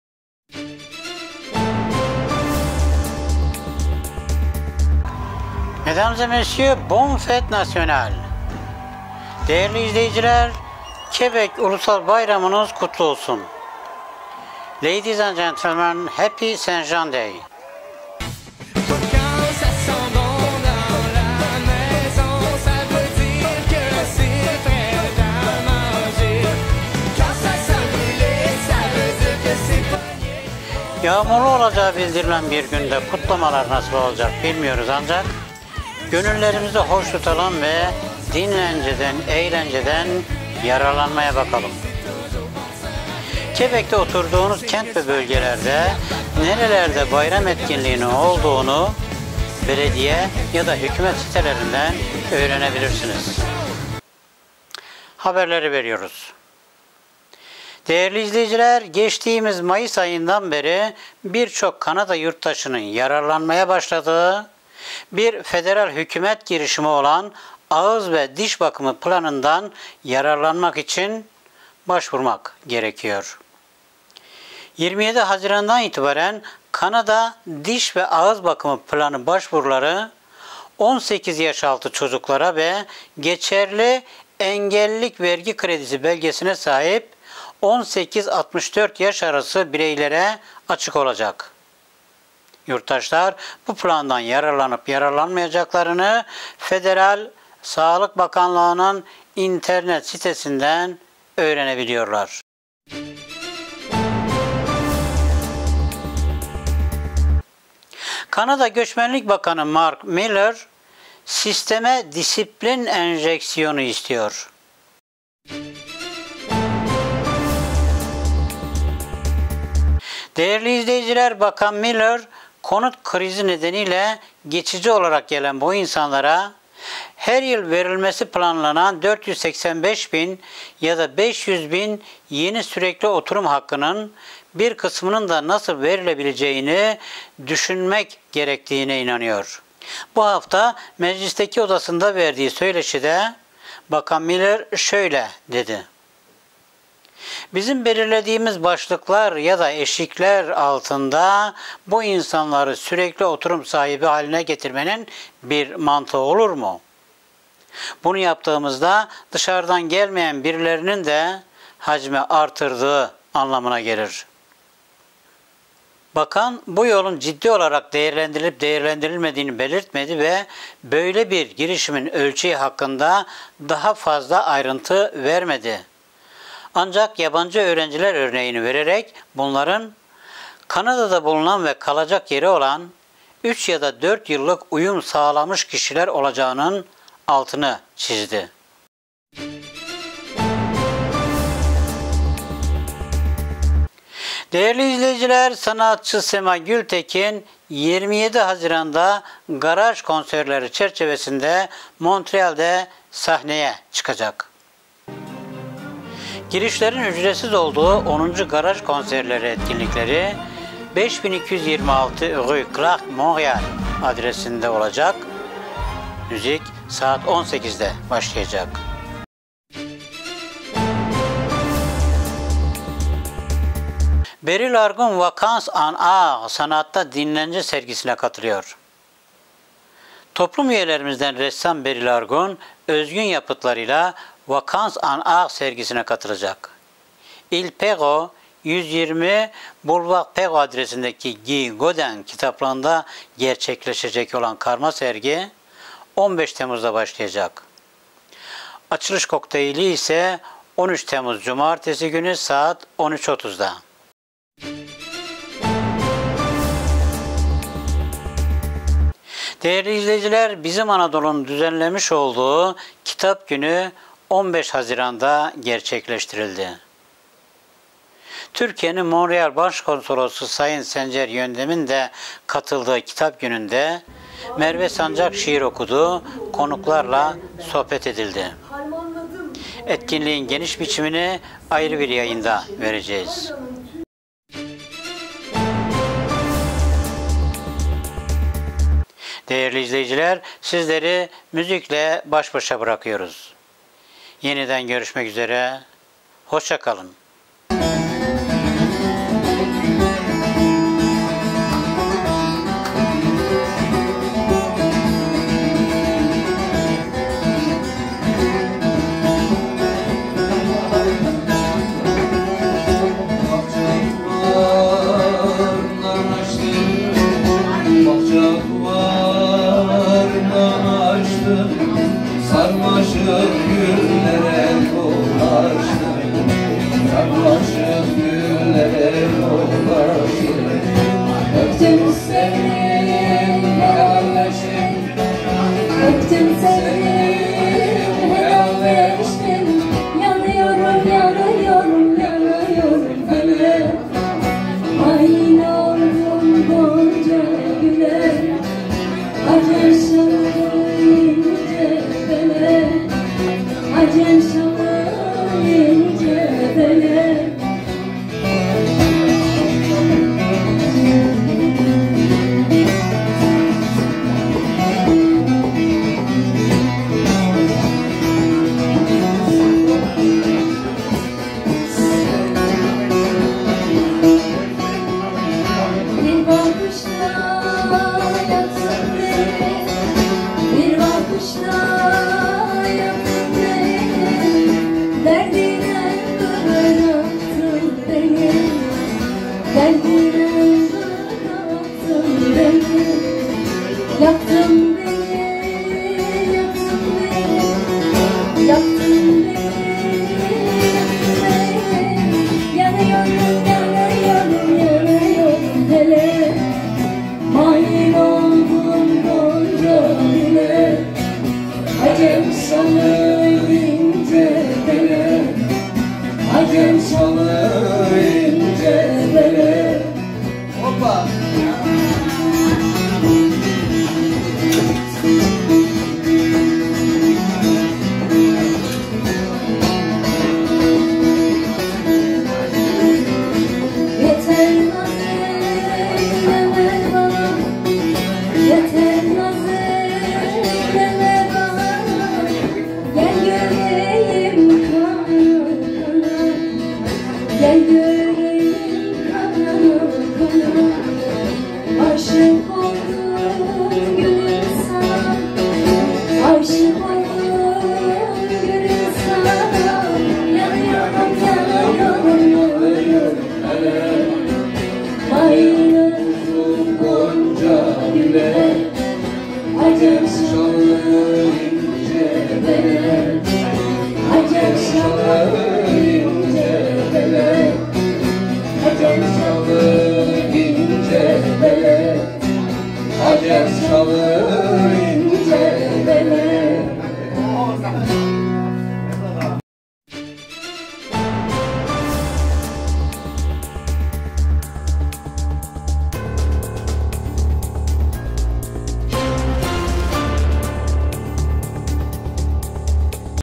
Ağız ve Diş Bakımı Planı Yürürlükte - Ömer F. Özen ile Kanada Haberleri - 24.06.2024 - Bizim Anadolu
Bonne fête nationale du Québec!
Kanada'da Ağız ve Diş Bakımı Planı yürürlükte
Kimler yararlanıyor?
Geçici Göçmenler Sürekli Oturum Hakkı alabilecek mi?
Sema Gültekin Garaj Konserlerinde
Beril Argun yapıtları Île Perrot'da
Kader Çolak ve Ergin Arslan İkilisinden Türkü Gecesi
Ve dahası...